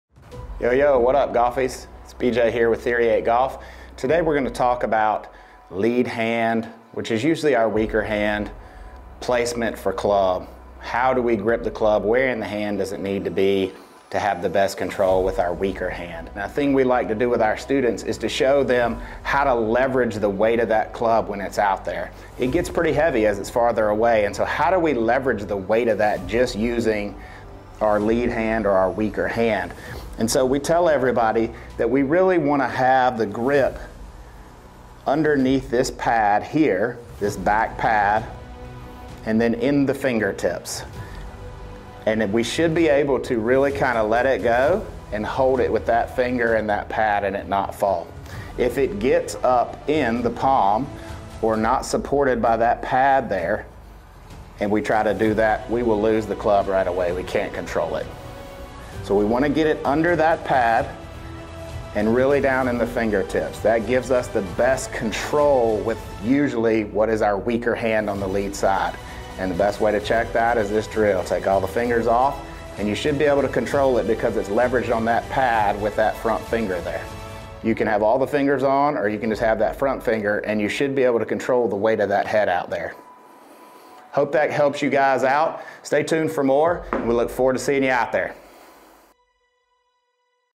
Lead Hand Leverage Is Key For Control
Gaining lead hand leverage gives us the ability to properly set the club in the backswing without using our trail arm, usually our dominate arm, to pull in an effort to help support the weight in the lift of the backswing which then can cause serious issues in transition, one of which is a loss of speed as well as face control at impact.  The ability to properly leverage the club in the lead hand is critical for setting the club properly on the backswing and giving us greater lag on the downswing for me speed and consistency.